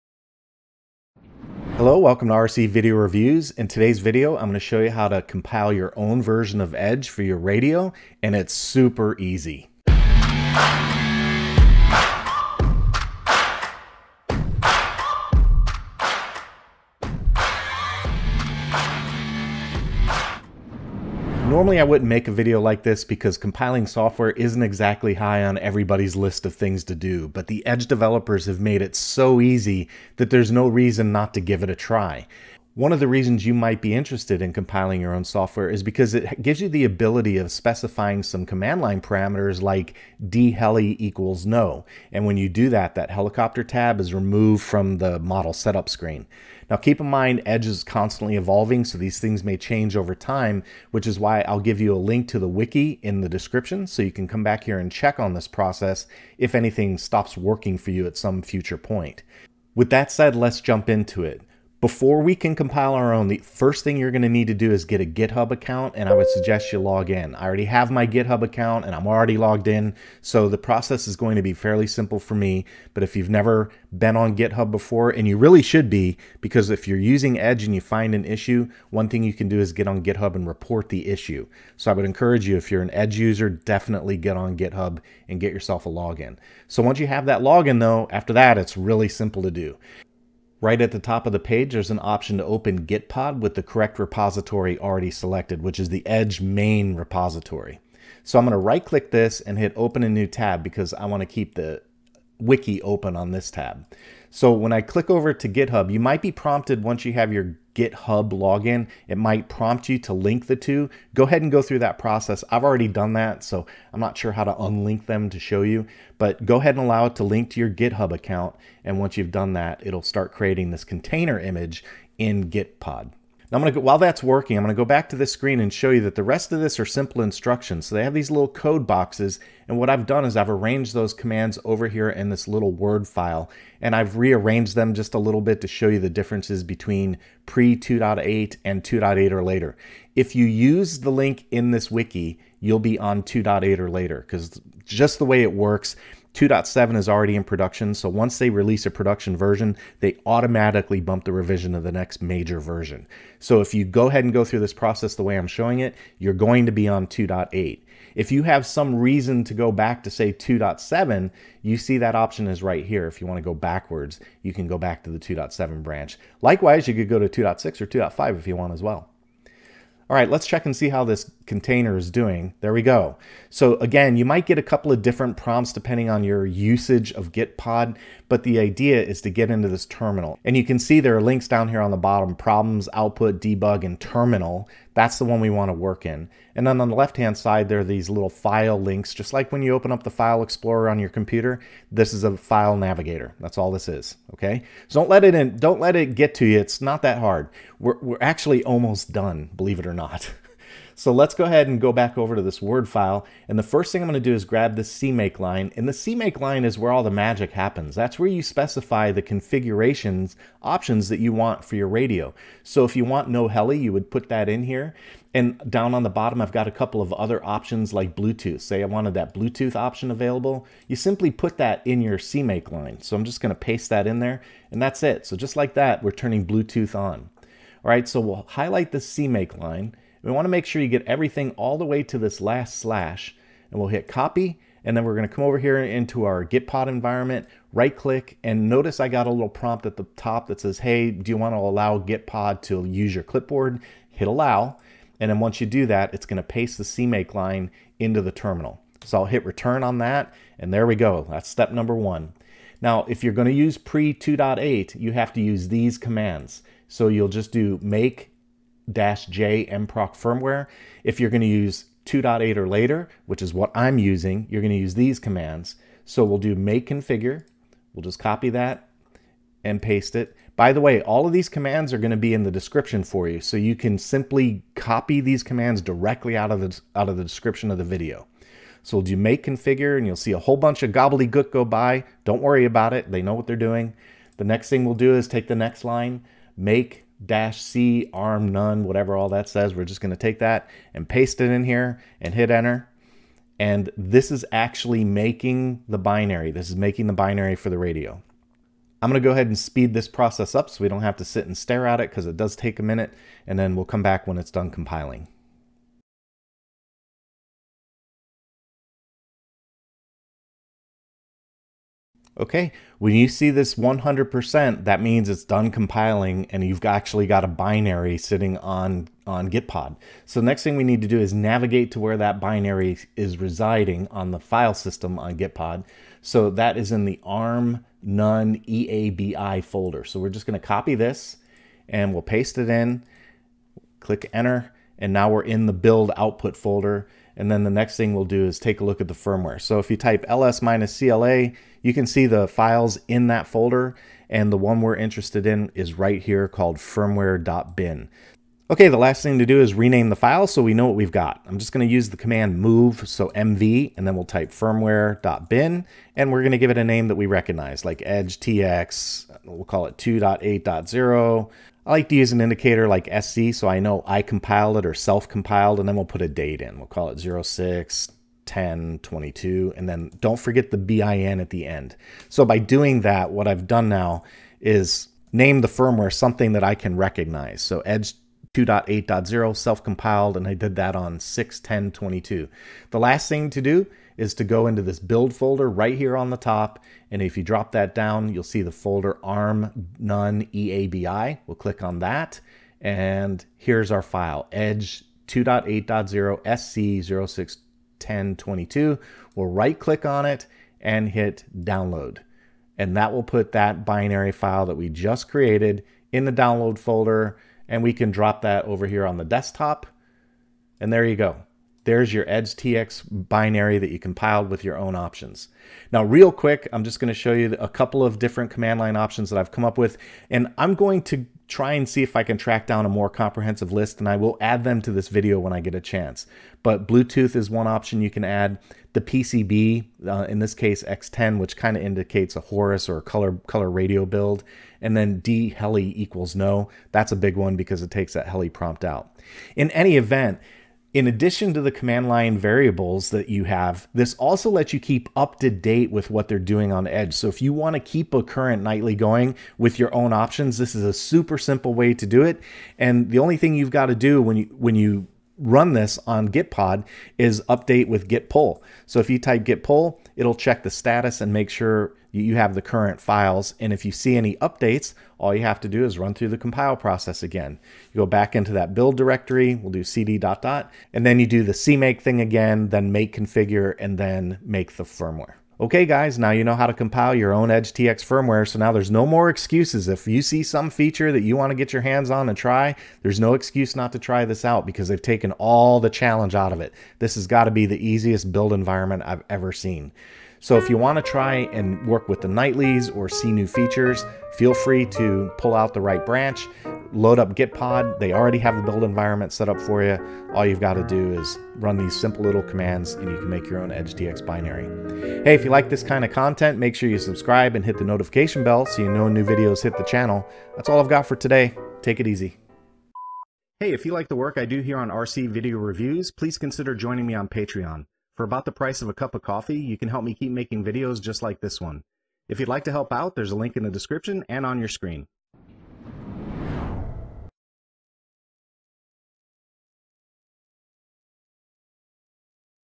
How-to Compile EdgeTX Yourself • ANYBODY Can Do This!
Our awesome EdgeTX team has enabled an insanely easy method for anybody to compile their own copy of the firmware using Gitpod. Essentially, they've taken all the work out of setting up a build environment. So if you can copy and paste, you can make your own EdgeTX firmware.

You might want to do this in order to specify your own options like "No Helicopter" tab. Or, you may want to keep up-to-speed with ongoing changes. Whatever your reason, watch this video and you'll see how simple it is to make EdgeTX yourself.

COMMANDS I USED IN THE VIDEO
cmake -Wno-dev -DPCB=X10 -DPCBREV=TX16S -DBLUETOOTH=YES -DDEFAULT_MODE=2 -DGVARS=YES -DPPM_UNIT=US -DHELI=NO -DCMAKE_BUILD_TYPE=Release ../

PRE 2.8:
make -j`nproc` firmware
mv firmware.bin edgetx_2.x.x_061022.bin

2.8 or LATER:
make configure
make -C arm-none-eabi -j`nproc` firmware
cd arm-none-eabi (omit for pre-2.8 versions)
mv firmware.bin edgetx_2.x.x_061022.bin

EXAMPLE CMAKE OPTIONS:
-DBLUETOOTH=YES
-DPCB=X10
-DHELI=NO
-DGVARS=NO